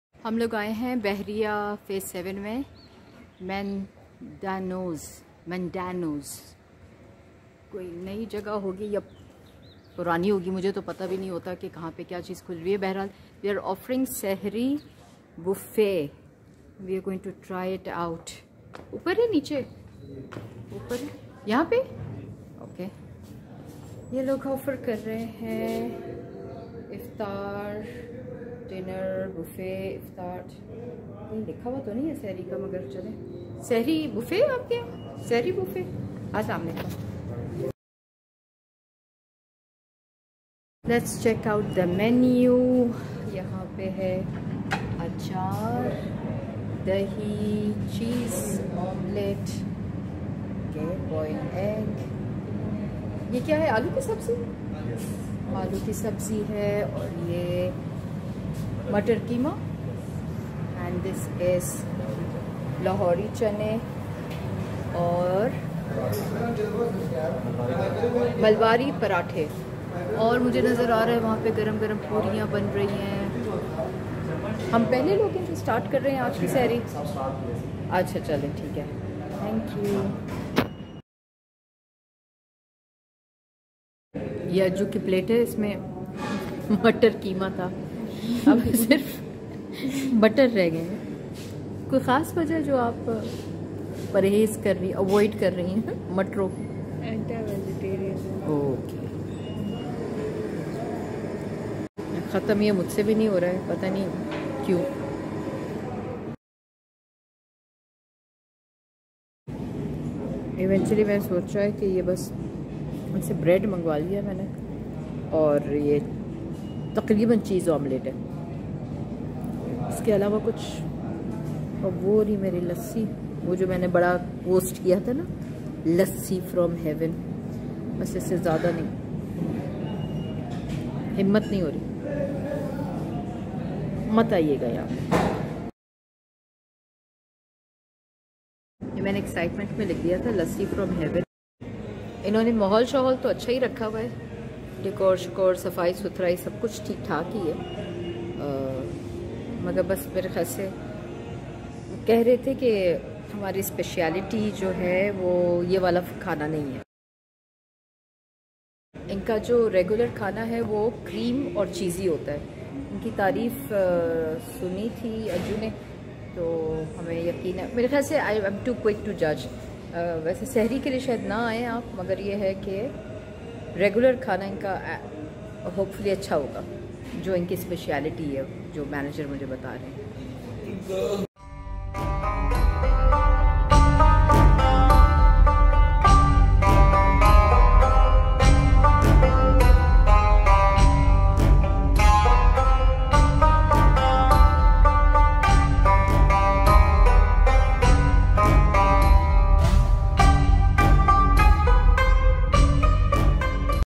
Sehri with Family | Lovely Time with 'Not to Recommend' Food | Vlog | Meera'n Malik
I shouldn't be judging too much. Will definitely visit them again after Eid for their regular food.